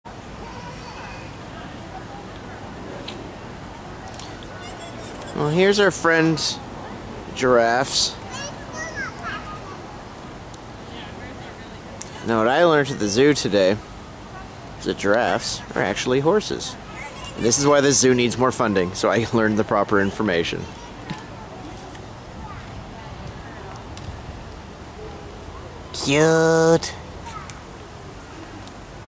California 2012 (Pt. 46) - Giraffes at the San Diego Zoo
February 11, 2012: The first animal we see at the San Diego zoo are the monstrously tall giraffes.